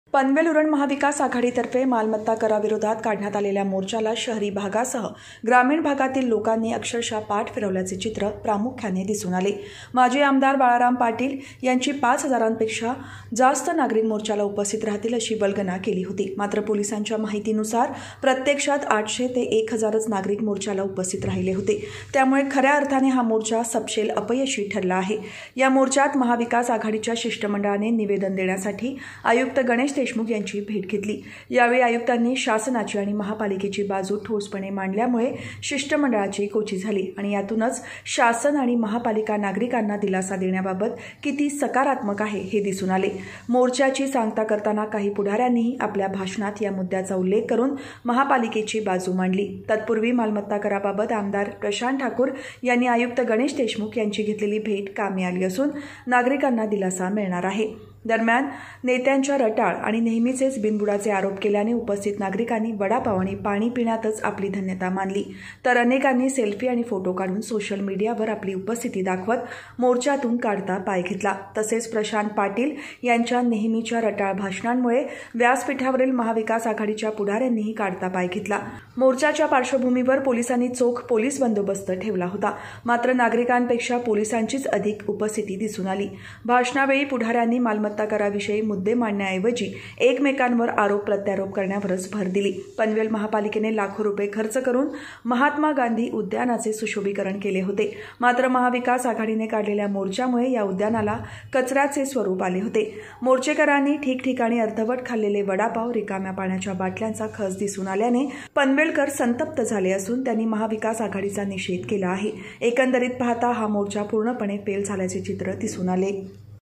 महाविकास आघाडीच्या मोर्च्याचा बोजवारा; नागरीकांनी फिरवली पाठ !
पनवेल उरण महाविकास आघाडीतर्फे मालमत्ता कराविरोधात काढण्यात आलेल्या मोर्चाला शहरी भागासह ग्रामीण भागातील लोकांनी अक्षरशः पाठ फिरवल्याचे चित्र दिसून आले. माजी आमदार बाळाराम पाटील यांनी पाच हजारांपेक्षा हजारापेक्षा जास्त नागरीक मोर्चाला उपस्थित राहतील अशी वल्गना केली होती, मात्र पोलिसांच्या माहितीनुसार प्रत्यक्षात केवळ आठशे ते हजार नागरिक मोर्च्याला उपस्थित राहिले होते. त्यामुळे हा मोर्चा खऱ्या अर्थाने सपशेल अपयशी ठरला आहे.